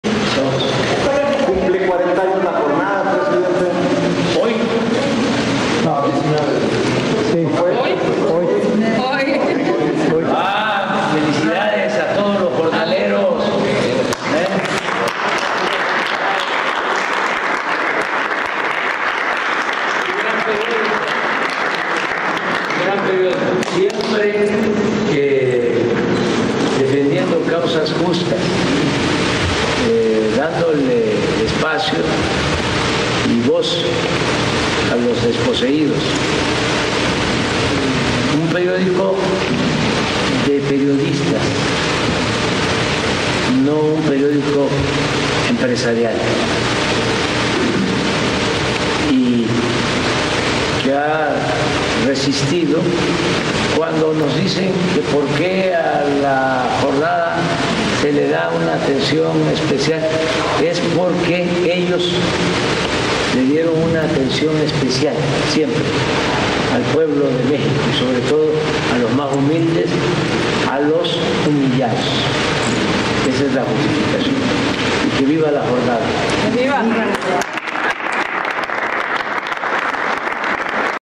López Obrador felicita a La Jornada por su 40 aniversario: "siempre defendiendo causas justas"
Andrés Manuel López Obrador felicita al equipo de trabajadores de La Jornada por su 40 aniversario:

"Siempre defendiendo causas justas y dándole espacio y voz a los desposeídos, un periódico de periodistas no uno empresarial", finalizó.